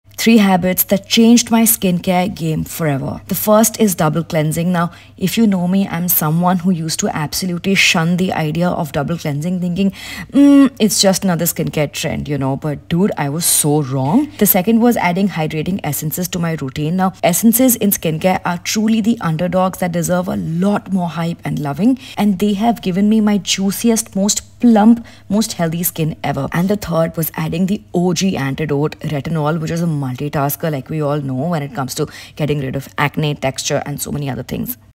3 Game Changing Skincare Habits!
3 habits that changed my skincare game forever! Save this 🙋‍♀️🌸

I started doing skincare around the mid of 2018 but really learnt to maximise it in 2021. Imagine it took me a full 3 year timeline to NAIL what works for me, how it works for me and why it works for me! Time and consistency is 🔑

In 2023, I’ve learnt a few more things about my skin and am trying to alter my routine around them 💦

Tell in the comments your game changing skincare habits 🤌